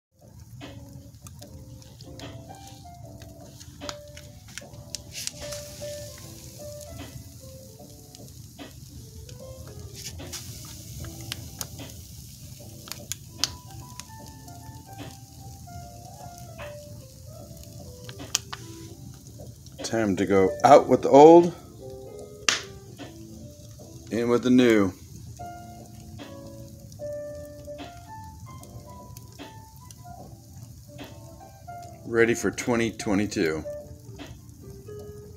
Letting go of past by Burning some of it; ready for a Fresh start 🔥in Gratitude
prepping for move before 2022; letting go of past 15+ years